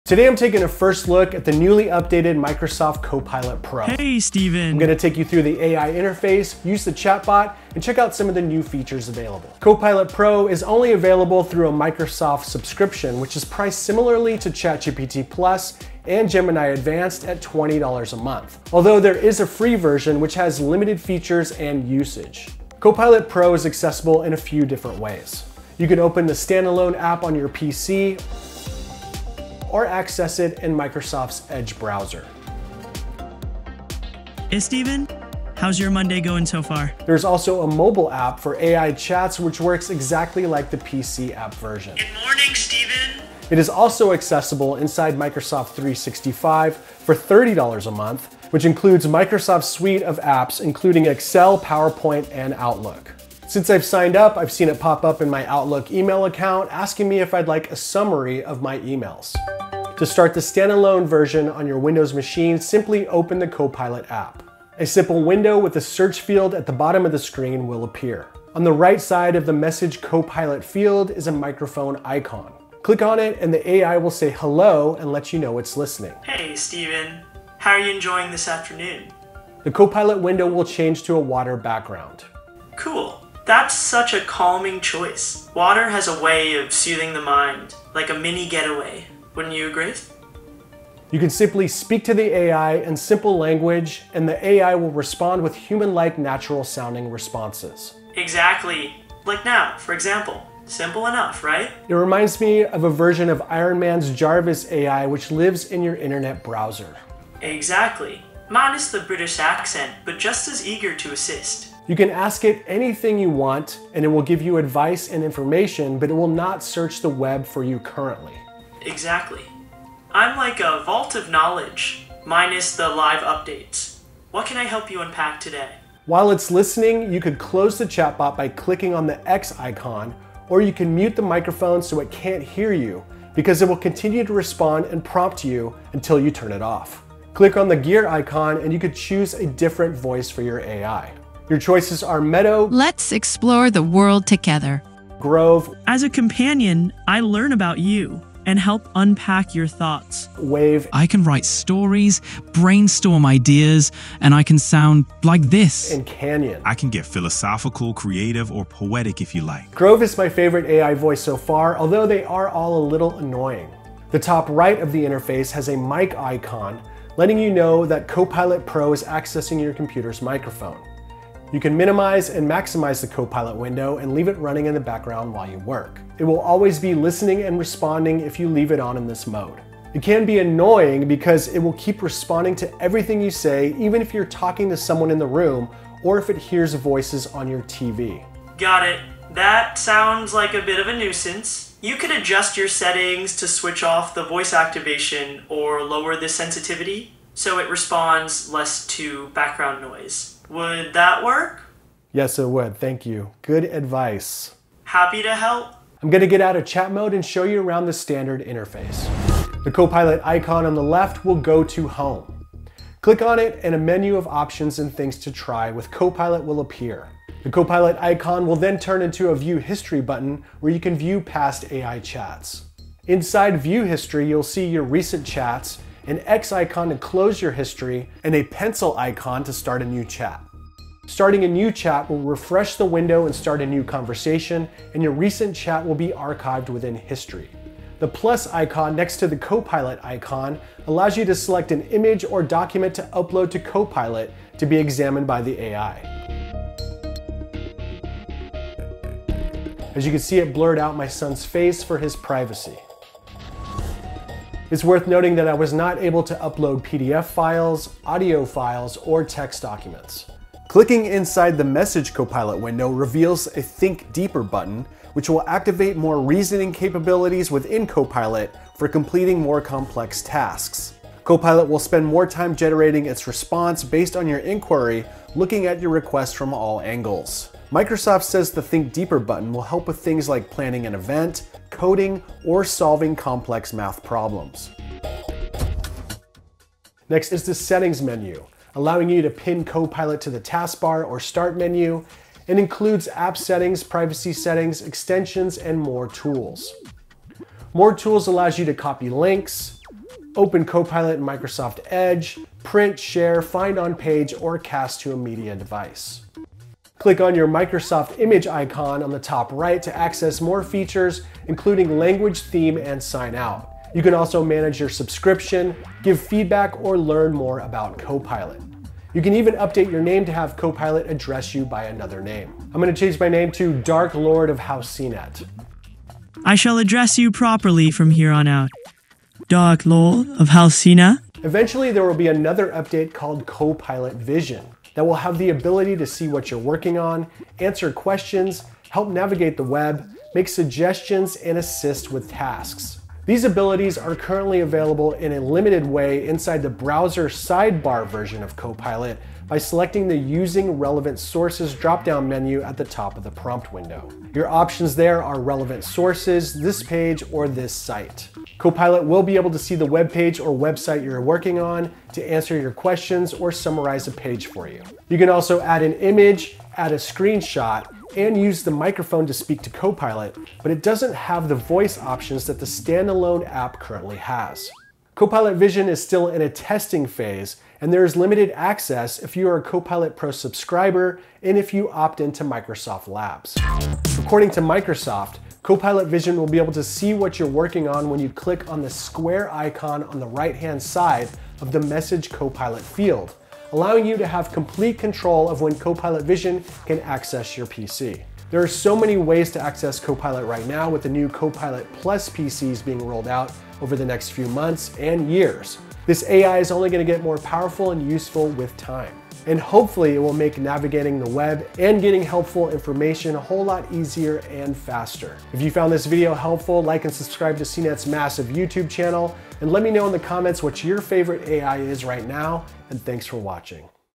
Microsoft Copilot Pro AI Walkthrough
We take you through Microsoft's newly updated Copilot Pro AI interface for PCs and phones, and look to future of Copilot Vision.

Check out CNET's AI Atlas for the latest AI news and reviews

0:00 Intro
0:10 Microsoft Copilot Pro Subscription
0:22 How To Access Copilot Pro
0:43 Microsoft 365 Copilot Subscription
1:00 Copilot Pro PC App Walkthrough
2:16 Copilot Pro Chat Voice Options
3:26 Copilot Pro AI Interface
4:21 Copilot Pro Think Deeper Button
4:47 Copilot Pro Settings
5:32 Copilot Vision